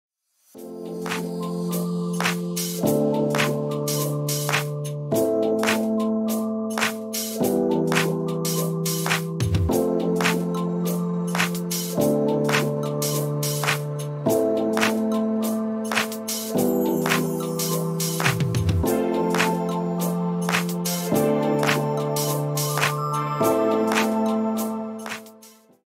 Where is your Gas Tank? | Car Tip Tuesday
Discover the hidden location of your vehicle's gas tank! Did you know that finding the gas tank can sometimes be tricky? Don't worry; we've got you covered. Follow @vehiclesforchange on YouTube and TikTok for expert car tips every Car Tip Tuesday. Learn how to locate your gas tank and never miss a refueling opportunity again! Stay informed and keep your ride running smoothly.